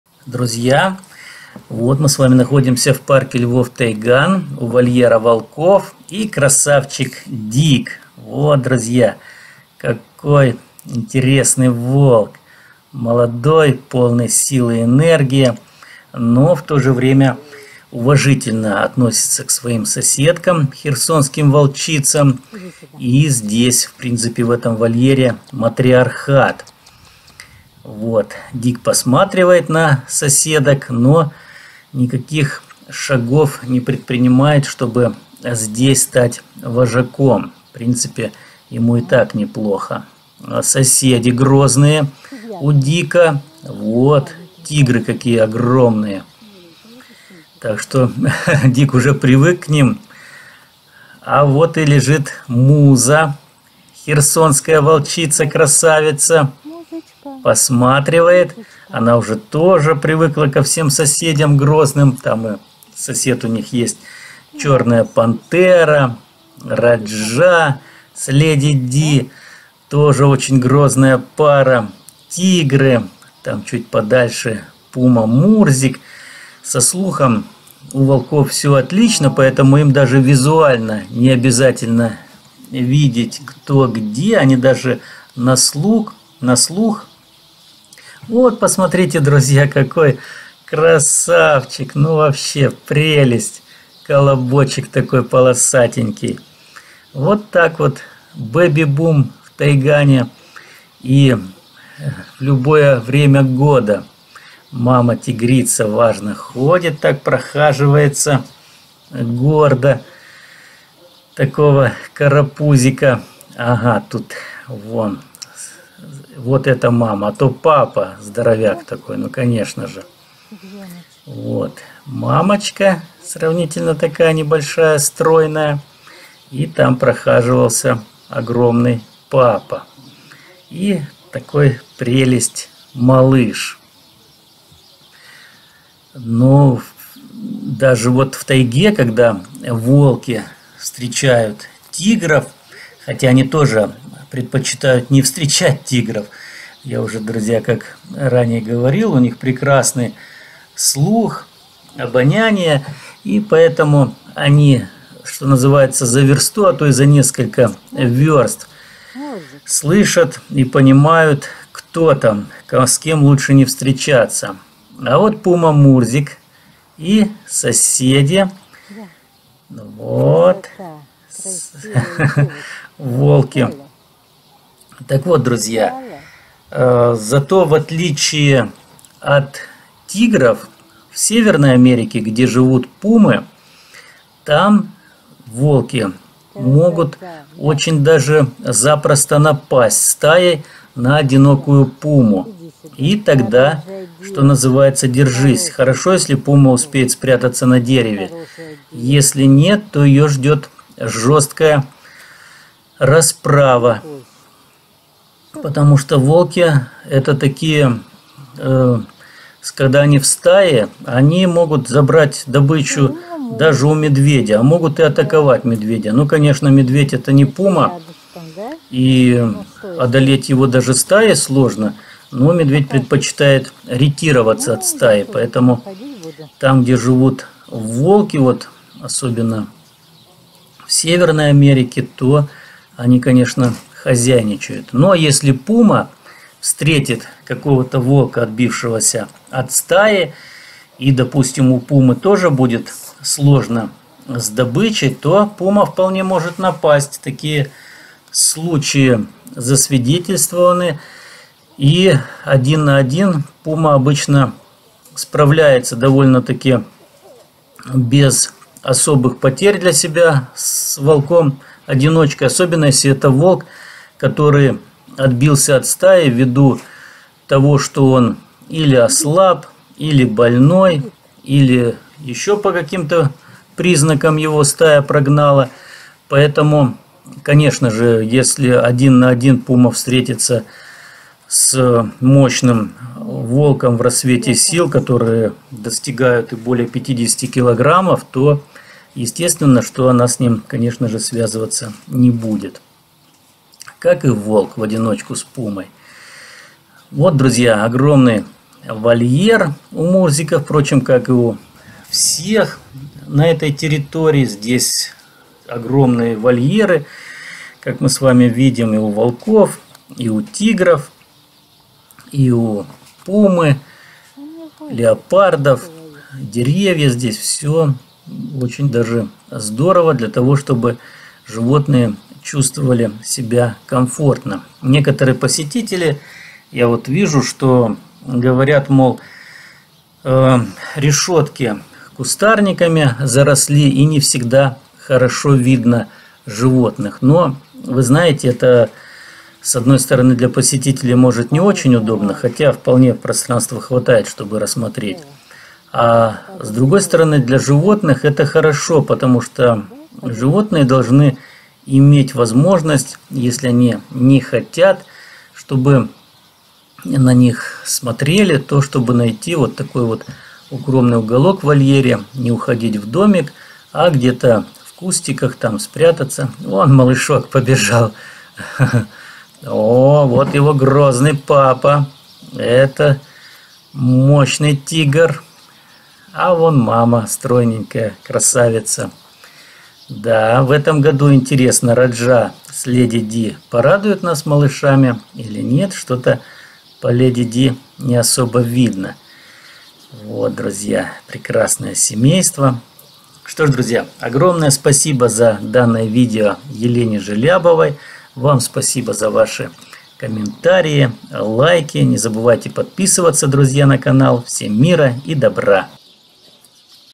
Семейство тигров. Волк Дик и его подружка Муза. Пума Мурзик.
Елена Желябова: Парк "Тайган"❤ Октябрь. Семейство тигров. Волк Дик и его подружка Муза. Пума Мурзик.